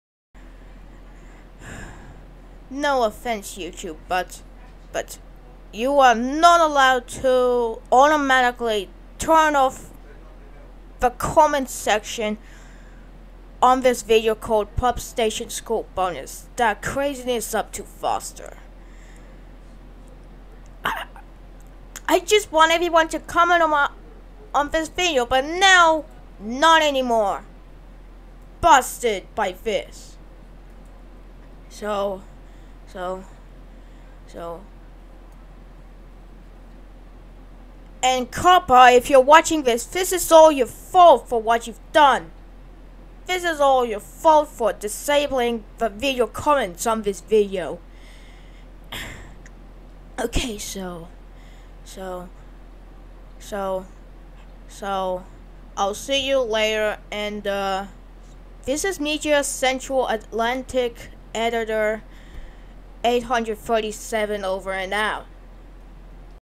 Never disable comments with my voice ever again, YouTube & COPPA! (serious message)
First Bandicam video on my new PC using my voice in the BG.